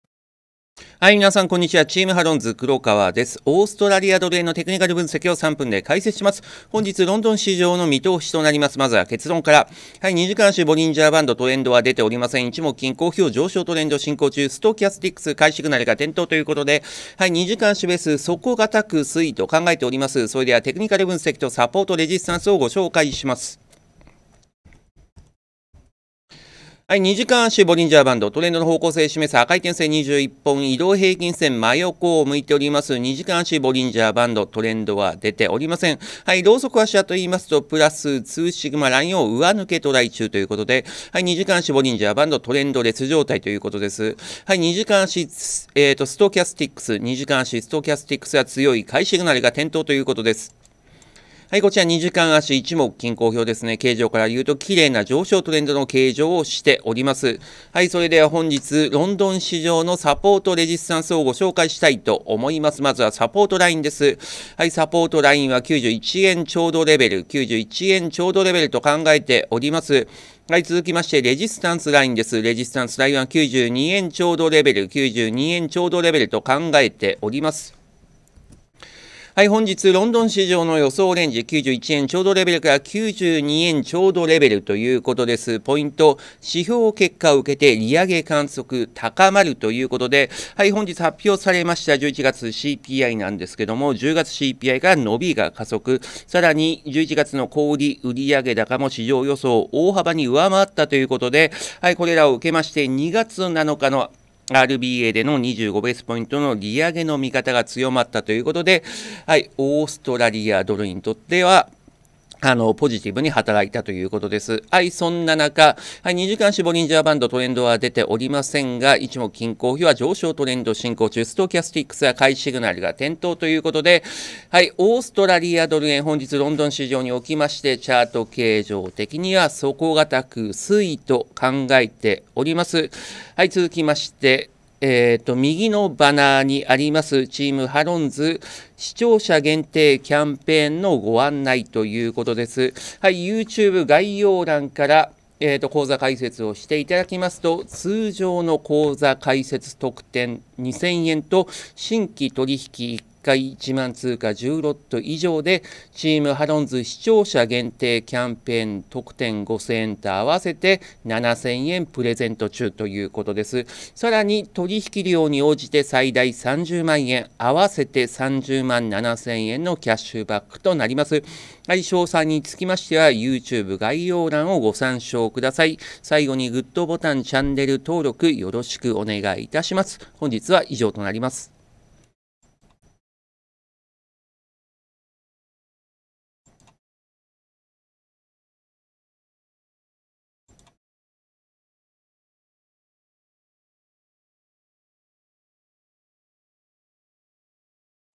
豪ドル/円見通し 「指標結果受けて利上げ観測高まる」見通しズバリ！3分テクニカル分析 ロンドン市場の見通し 2023年1月11日
★☆★ TEAMハロンズ視聴者限定キャンペーン！ ★☆★